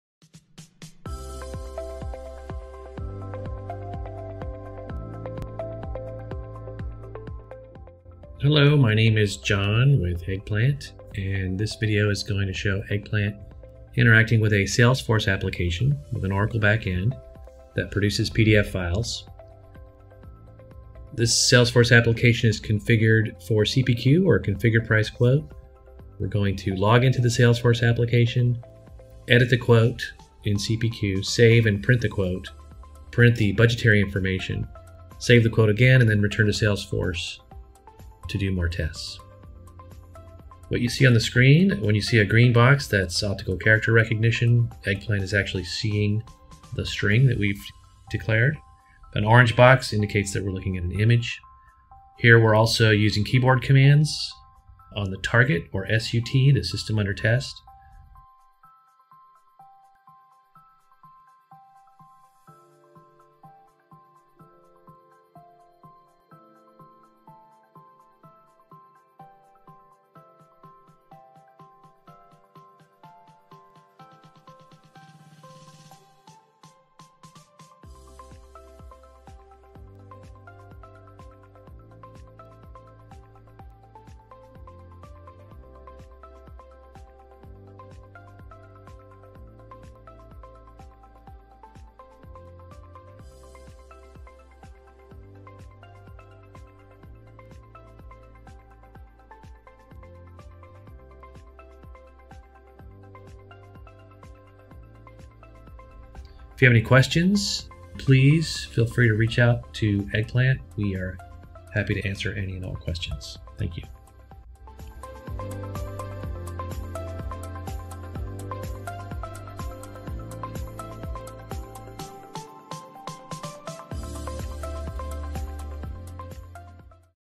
Automating Salesforce & CPQ with Eggplant
In this video, Eggplant automates the process for editing and printing quotes in a Salesforce application with an Oracle backend configured with Configure Price Quote (CPQ).